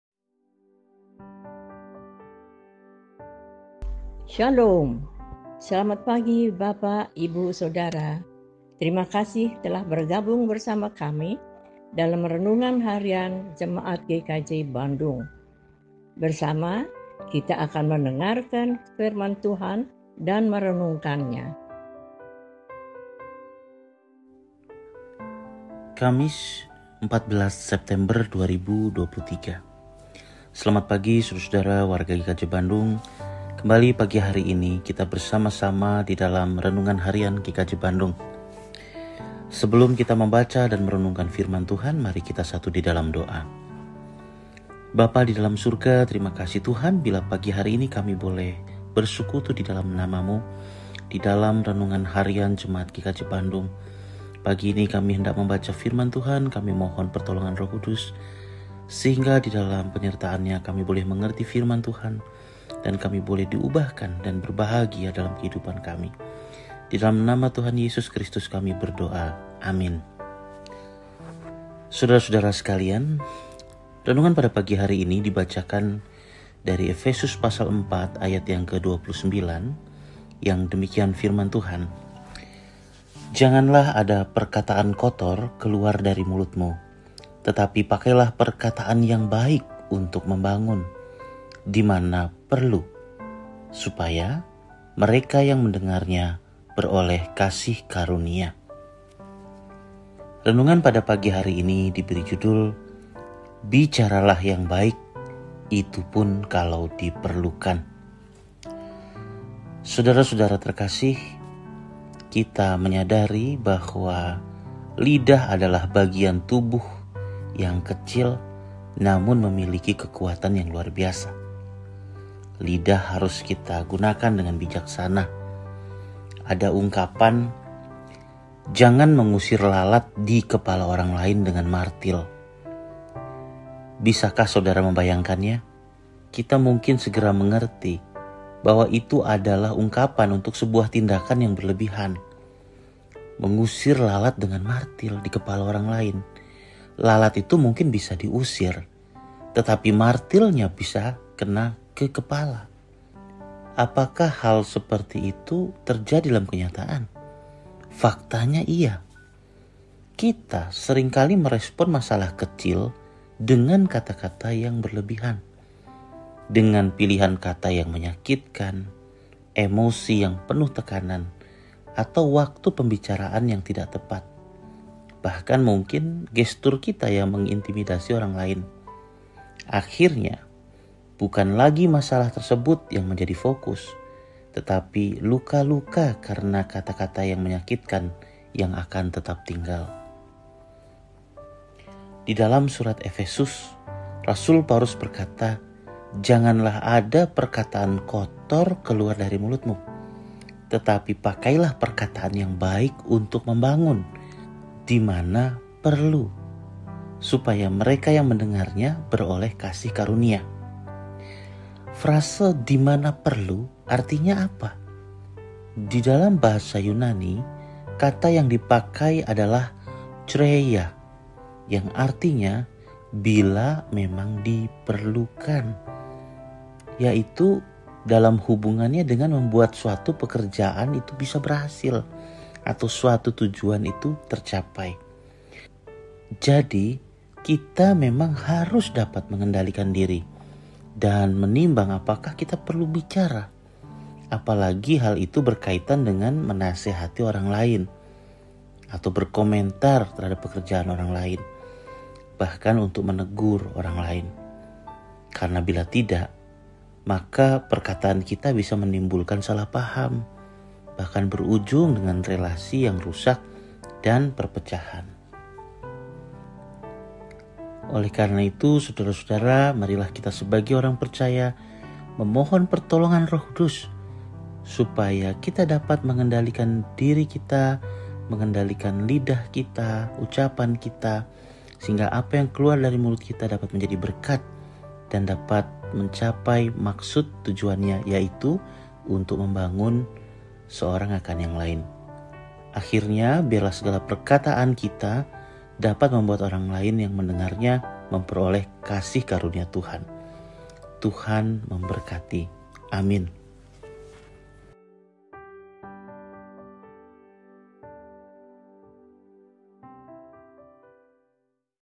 Renungan Harian 14 September 2023
Tema : BICARALAH YANG BAIK…ITUPUN KALAU DIPERLUKAN
Oleh : Pdt. Daru Marhaendhy